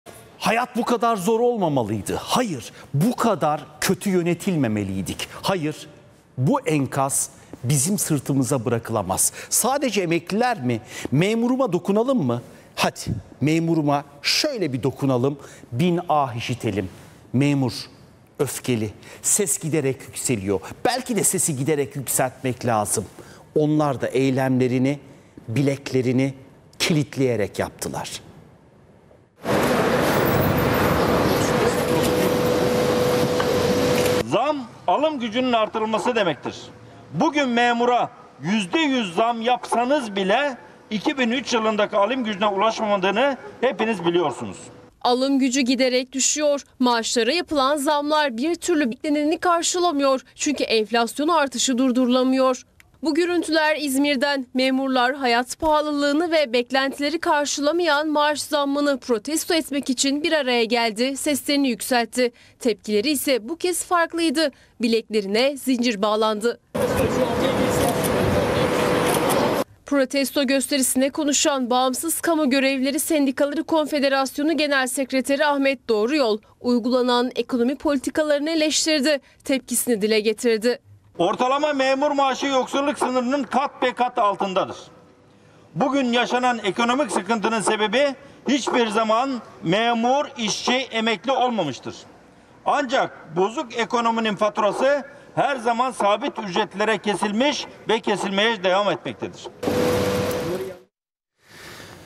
Memurlardan yetersiz maaş zamları için gündeme bomba gibi düşecek zincirli protesto!
Memurlardan yetersiz maaş zammı için gündeme bomba gibi düşecek zincirli protesto!